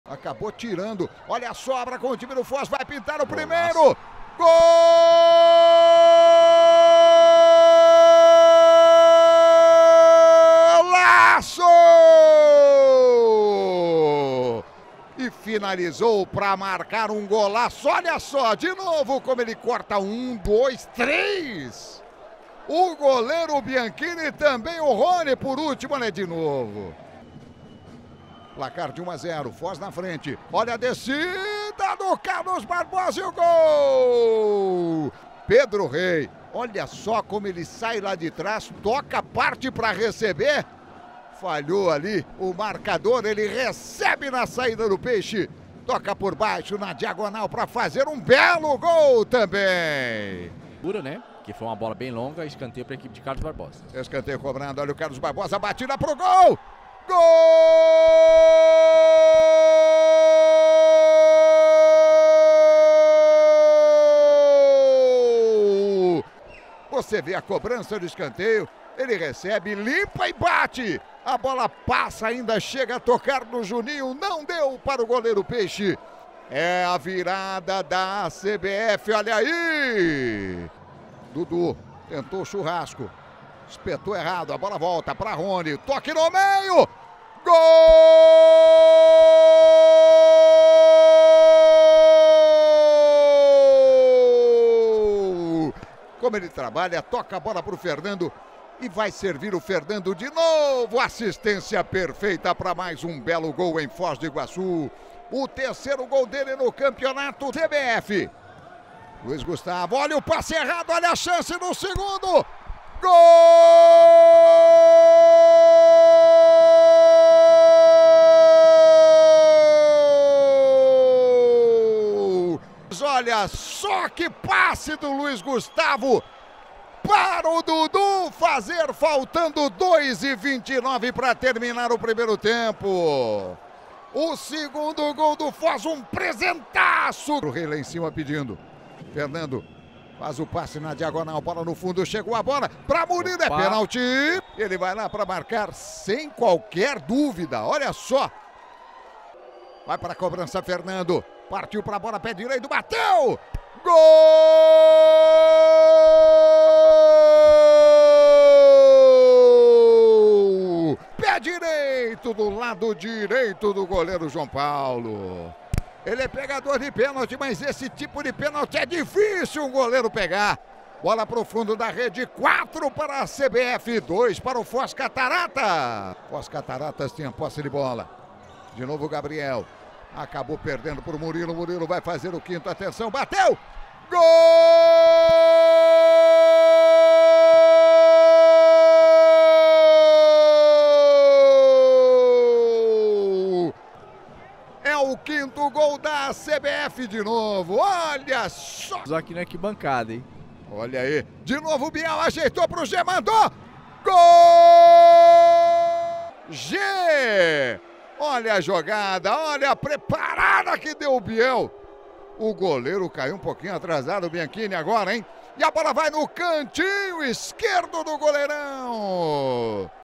LNF 2022 - 14ª RODADA - GOLS - FOZ 3 X 5 ACBF
Gols:
Foz Cataratas - Raul, Dudu e Gê
ACBF - Pedro Rei, Pedrinho, Fernando (2) e Murilo